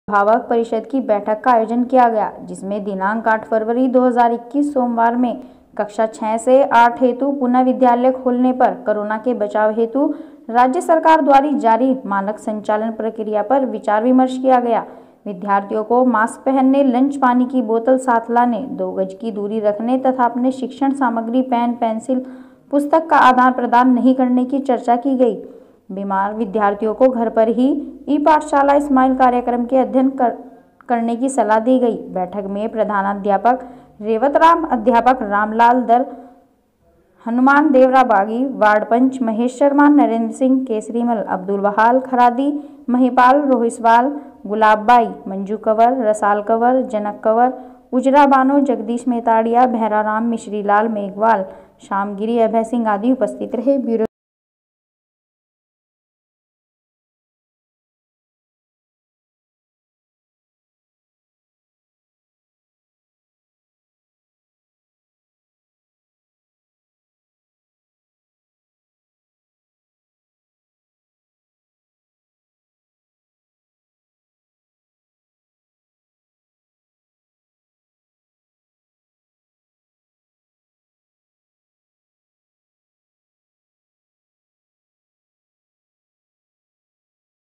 पाली राजकीय माध्यमिक विद्यालय देवली हुल्ला में रविवार को अध्यापक अभिभावक परिषद की बैठक का आयोजन
पी टी एम बैठक में हुई कोरोना से बचाव हेतु चर्चा *

 पाली क्षेत्र के देवली हुल्ला के राजकीय माध्यमिक विद्यालय देवली हुल्ला में रविवार को अध्यापक अभिभावक परिषद की बैठक का आयोजन किया गया ।जिसमें दिनांक 8 फरवरी 2021 सोमवार से कक्षा 6 से 8 हेतु पुनः विद्यालय खुलने पर कोरोना से बचाव हेतु राज्य सरकार द्वारा जारी मानक संचालन प्रक्रिया पर विचार विमर्श किया गया। विद्यार्थियों को मास्क पहनने ,लंच पानी की बोतल साथ लाने ,2 गज की दूरी रखने तथा अपने शिक्षण सामग्री पेन, पेंसिल पुस्तक का आदान-प्रदान नहीं करने पर चर्चा की ।बीमार विद्यार्थियों को घर पर ही ई पाठशाला स्माइल कार्यक्रम से अध्ययन करने की सलाह दी। बैठक में प्रधानाध्यापक रेवतराम, अध्यापक रामलाल दरठ हनुमान देवरा बगडी  ,वार्ड पंच महेश शर्मा, नरेंद्र सिंह ,केशरी मल, अब्दुल वहाब खरादी ,महिपाल रोहिसवाल ,गुलाब बाई मीणा, मंजू कवर, रसाल कंवर,जनक कवर, ऊजरा बानो ,जगदीश मेड़तिया ,भैराराम ,मिश्री लाल मेघवाल ,श्याम गिरी, अभय सिंह आदि  उपस्थित थे।

रिपोर्ट कालु भाई खजवाणिया सियाट सोजत